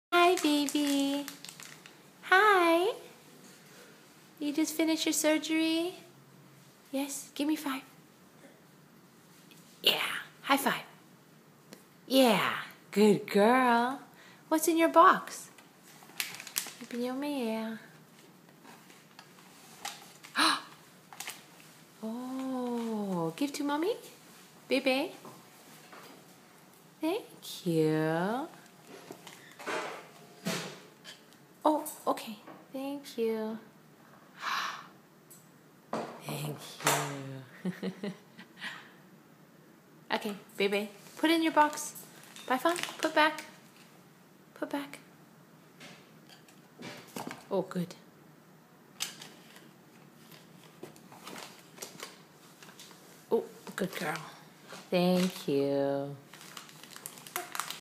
Baby Lauren after surgery
Amazing how adaptable kids are! Just hours after a procedure to drain and remove the lump on her neck, Lauren is sitting up and playing with one hand (the other hand has her IV in it).       At this time, her face (and body) are still a bit swollen.